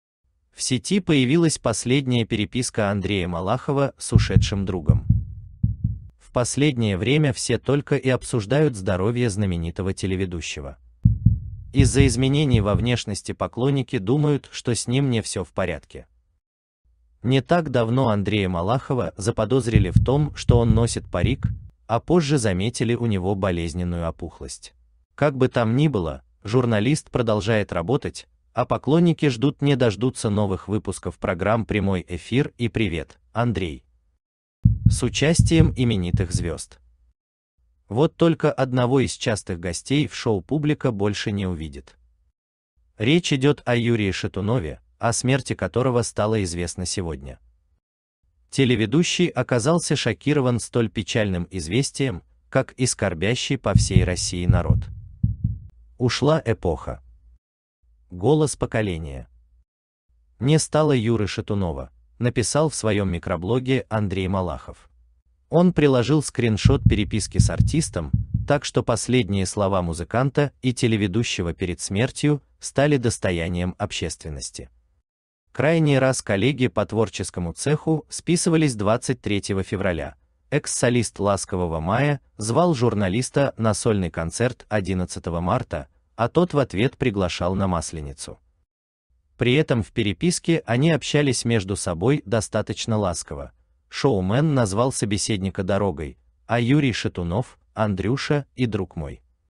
Шок! Последняя переписка Юрия Шатунова попала в сеть
В Сети появилась последняя переписка Андрея Малахова с ушедшим другом.

В сети появилось видео, на котором запечатлены последние минуты жизни экс-солиста популярной советской группы "Ласковый май" Юрия Шатунова. Кумир молодежи 90-х скончался ночью 23 июня от обширного инфаркта по пути в больницу.